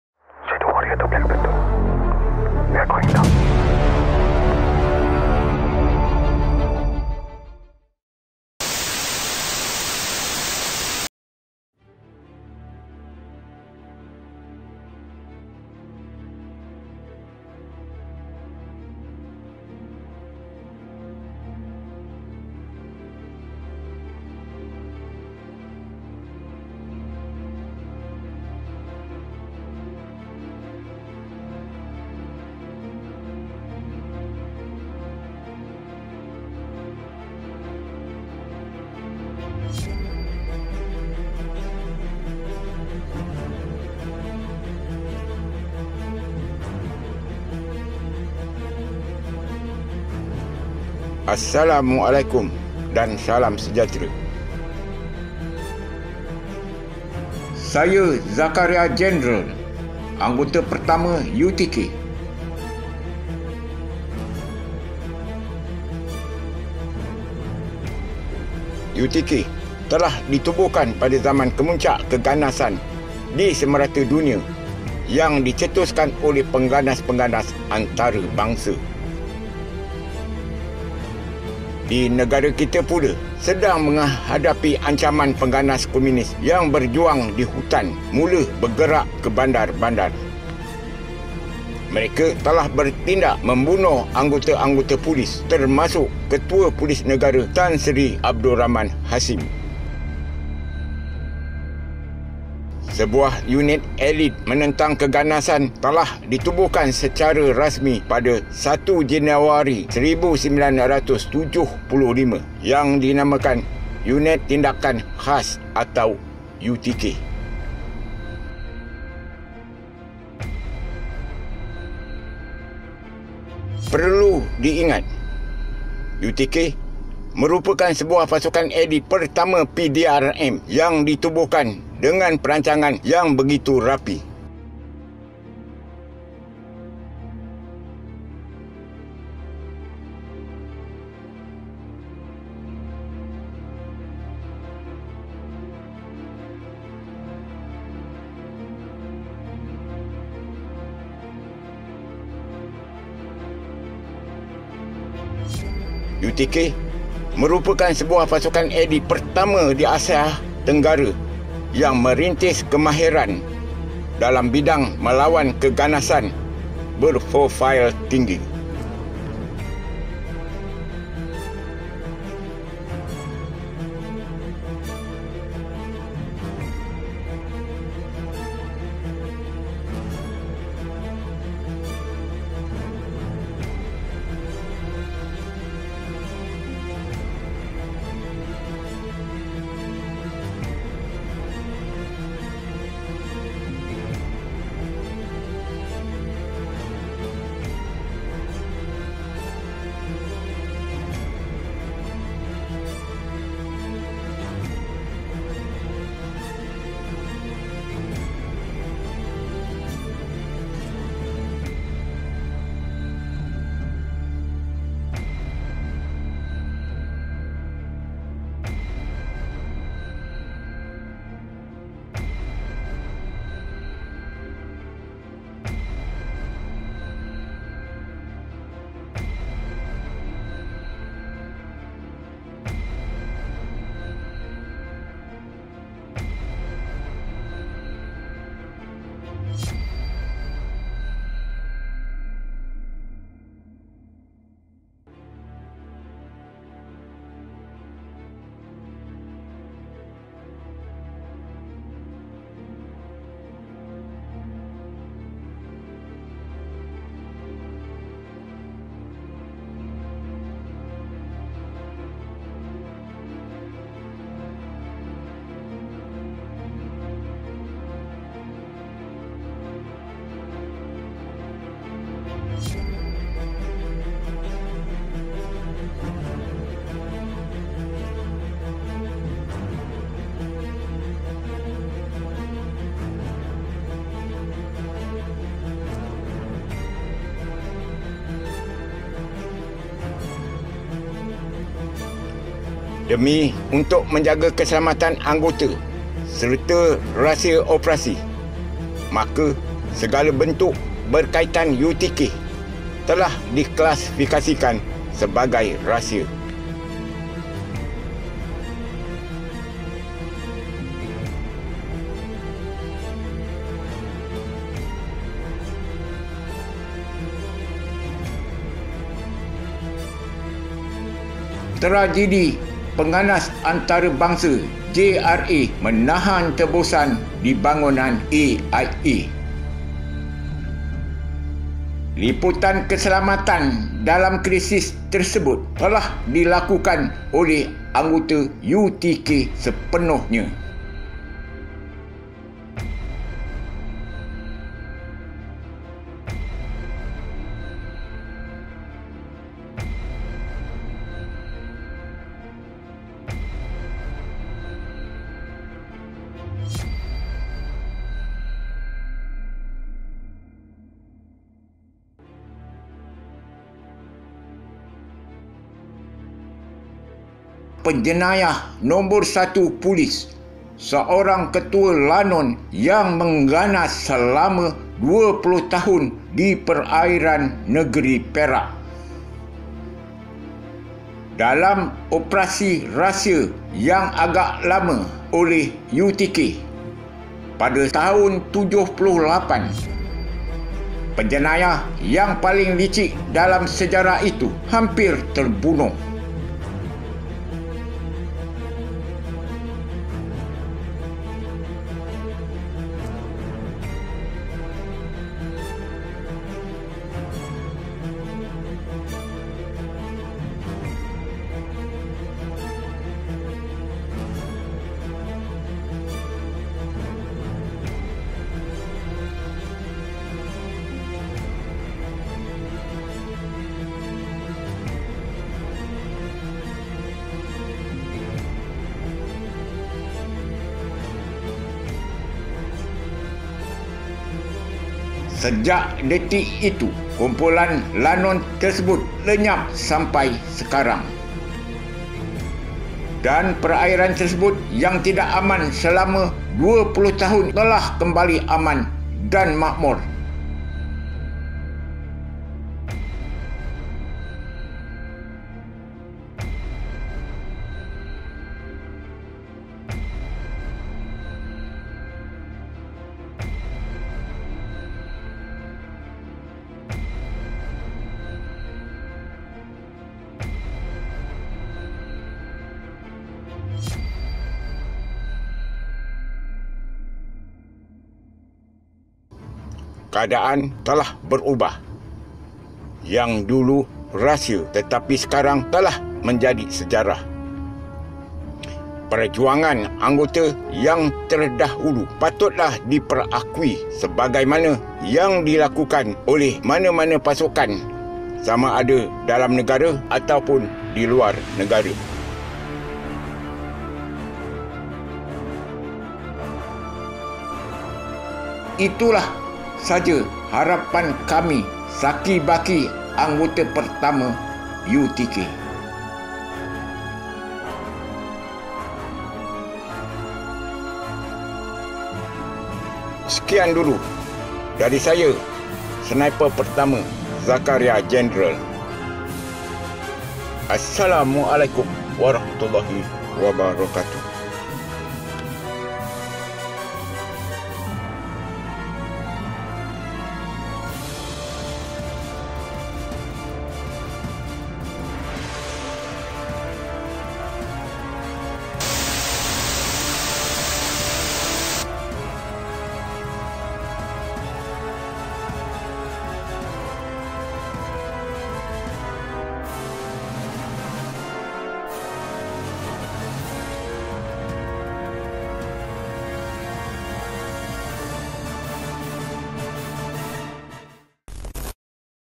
Galeri Sejarah 'Maya' Unit Tindakan Khas - Selamat HUT U.T.K ke 47
Maaf atas kelewatan publish. (menunggu approval dari Youtube mmg begitu).

Untuk tahun baru 2022 ini, MSW membawakan sekali lagi 'walkthrough' Galeri Sejarah 'Maya' Unit Tindakan Khas - yang memfokuskan pada segmen awal penubuhan UTK sebagai unit elit PDRM.

Tuan Zakaria General akan menemani anda sepanjang pameran ini dan anda boleh menekan 'pause' di mana-mana bahagian video jika berminat untuk membaca lebih lanjut pada info panel yang kami sediakan dalam pameran galeri kali ini.

Selamat HUT U.T.K ke 47

Nota :
Ini hanyalah galeri maya, pameran tidak mengikut garis panduan keselamatan pengendalian pameran senjata yang diamalkan oleh organisasi keselamatan. Hanya sekadar hiburan maya.

Dapatkan naskah terhad catatan memoir Tuan Zakaria General ~ "UTK ANTARA RAHSIA & SEJARAH" yang kini boleh dimiliki melalui link SHOPEE dibawah: